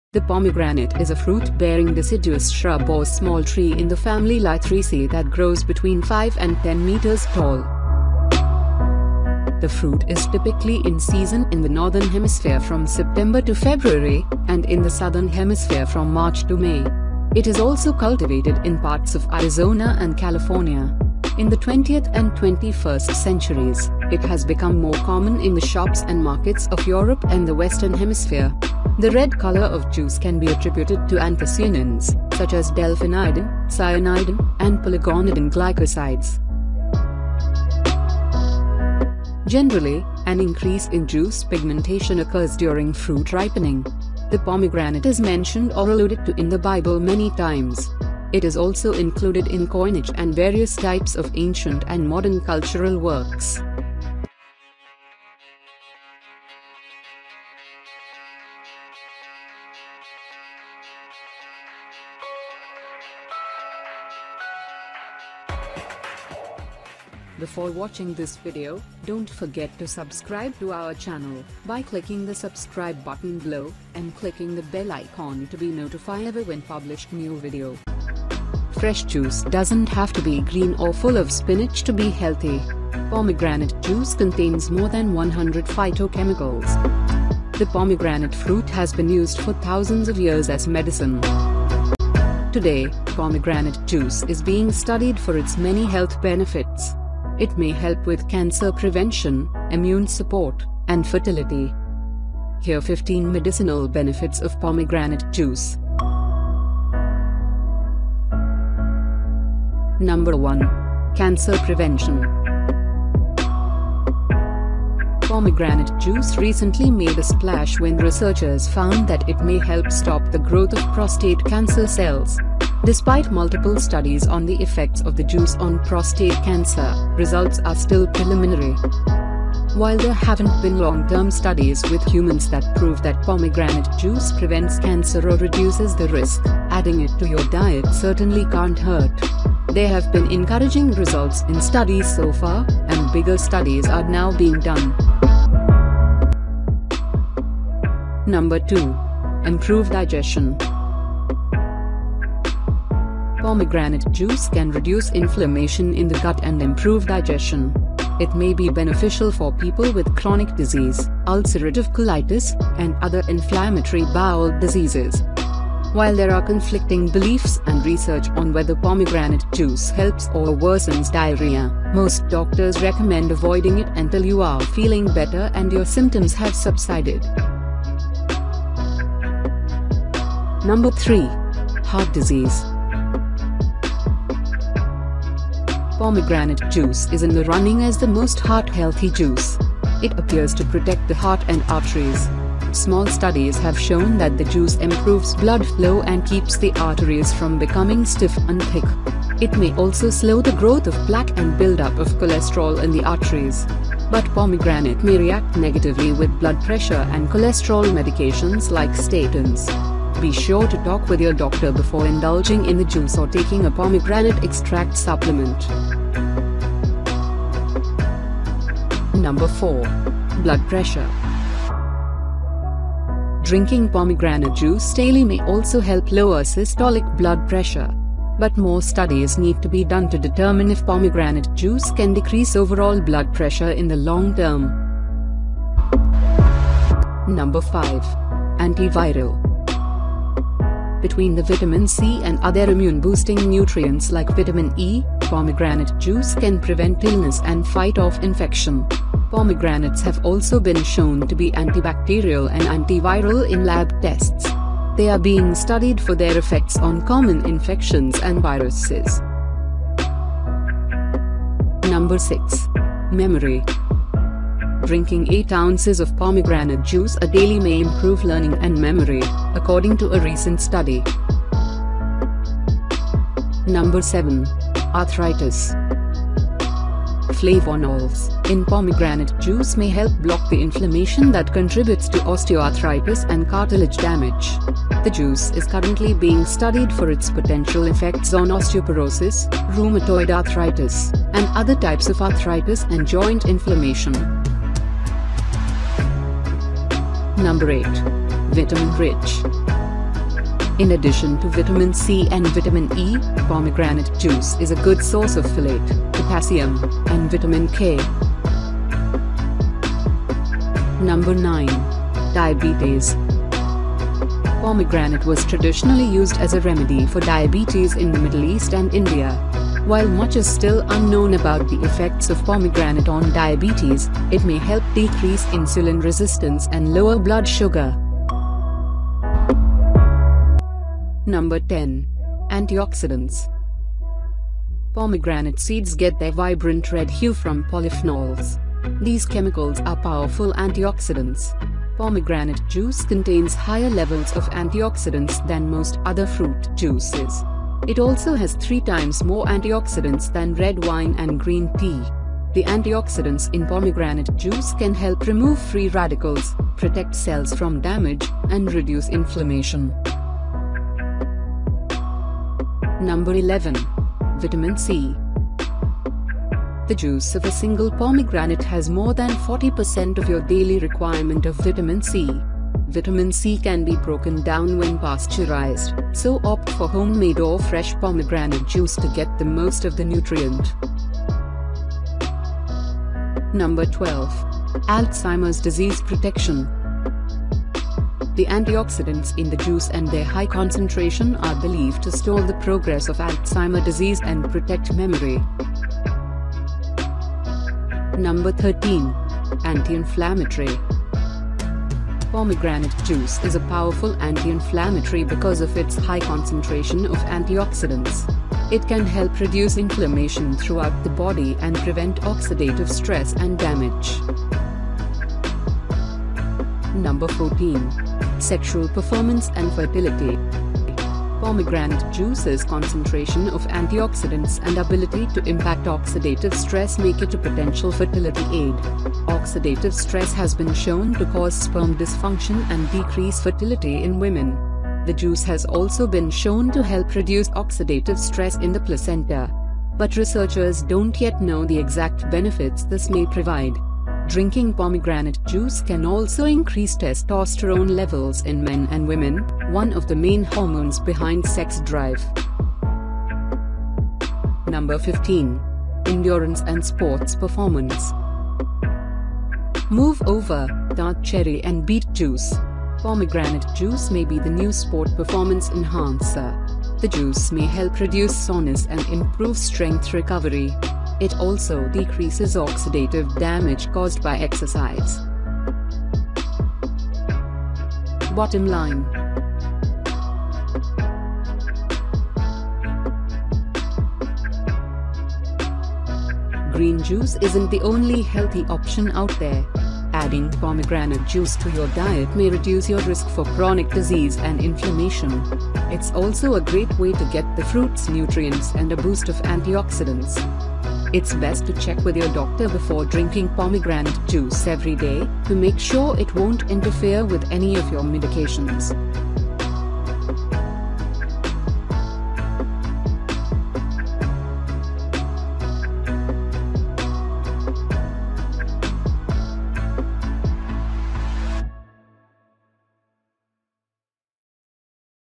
Just 1 Juice will Unclog Arteries, lower blood pressure, Diabetes, lose weight And Many more!
Do You Have Any Idea What Will Happen If You Drink Pomegranate Juice every day?

The pomegranate is a fruit-bearing deciduous shrub or small tree in the family Lythraceae that grows between 5 and 10 m  tall. The fruit is typically in season in the Northern Hemisphere from September to February, and in the Southern Hemisphere from March to May. It is also cultivated in parts of Arizona and California.  In the 20th and 21st centuries, it has become more common in the shops and markets of Europe and the Western Hemisphere. The red color of juice can be attributed to anthocyanins, such as delphinidin, cyanidin, and pelargonidin glycosides. Generally, an increase in juice pigmentation occurs during fruit ripening. The pomegranate is mentioned or alluded to in the Bible many times. It is also included in coinage and various types of ancient and modern cultural works.

Fresh juice doesn’t have to be green or full of spinach to be healthy. Pomegranate juice contains more than 100 phytochemicals. The pomegranate fruit has been used for thousands of years as medicine.
Today, pomegranate juice is being studied for its many health benefits. It may help with cancer prevention, immune support, and fertility.
Here 15 Medicinal benefits of pomegranate juice.
Number 1. Cancer prevention.
Pomegranate juice recently made a splash when researchers found that it may help stop the growth of prostate cancer cells. Despite multiple studies on the effects of the juice on prostate cancer, results are still preliminary.
While there haven’t been long-term studies with humans that prove that pomegranate juice prevents cancer or reduces the risk, adding it to your diet certainly can’t hurt. There have been encouraging results in studies so far, and bigger studies are now being done.

Number 2. improve Digestion.
Pomegranate juice can reduce inflammation in the gut and improve digestion. It may be beneficial for people with Crohn’s disease, ulcerative colitis, and other inflammatory bowel diseases.
While there are conflicting beliefs and research on whether pomegranate juice helps or worsens diarrhea, most doctors recommend avoiding it until you are feeling better and your symptoms have subsided.

Number 3. Heart disease.
Pomegranate juice is in the running as the most heart-healthy juice. It appears to protect the heart and arteries. Small studies have shown that the juice improves blood flow and keeps the arteries from becoming stiff and thick. It may also slow the growth of plaque and buildup of cholesterol in the arteries. But pomegranate may react negatively with blood pressure and cholesterol medications like statins. Be sure to talk with your doctor before indulging in the juice or taking a pomegranate extract supplement.

Number 4. Blood pressure.
Drinking pomegranate juice daily may also help lower systolic blood pressure. But more studies need to be done to determine if pomegranate juice can decrease overall blood pressure in the long term.
Number 5. Antiviral.
Between the vitamin C and other immune-boosting nutrients like vitamin E, pomegranate juice can prevent illness and fight off infection. Pomegranates have also been shown to be antibacterial and antiviral in lab tests. They are being studied for their effects on common infections and viruses.

Number 6. Memory.
Drinking 8 ounces of pomegranate juice a daily may improve learning and memory, according to a recent study.
Number 7. Arthritis.
Flavonols in pomegranate juice may help block the inflammation that contributes to osteoarthritis and cartilage damage. The juice is currently being studied for its potential effects on osteoporosis, rheumatoid arthritis, and other types of arthritis and joint inflammation.
Number 8. Vitamin-rich.
In addition to vitamin C and vitamin E, pomegranate juice is a good source of folate, potassium, and vitamin K.
Number 9. Diabetes.
Pomegranate was traditionally used as a remedy for diabetes in the Middle East and India. While much is still unknown about the effects of pomegranate on diabetes, it may help decrease insulin resistance and lower blood sugar.

▬▬▬▬▬▬▬▬ஜ۩۞۩ஜ▬▬▬▬▬▬▬▬▬
▬▬▬▬▬▬▬▬ஜ۩۞۩ஜ▬▬▬▬▬▬▬▬▬

▬▬▬▬▬▬▬▬ஜ۩۞۩ஜ▬▬▬▬▬▬▬▬▬

▬▬▬▬▬▬▬▬ஜ۩۞۩ஜ▬▬▬▬▬▬▬▬▬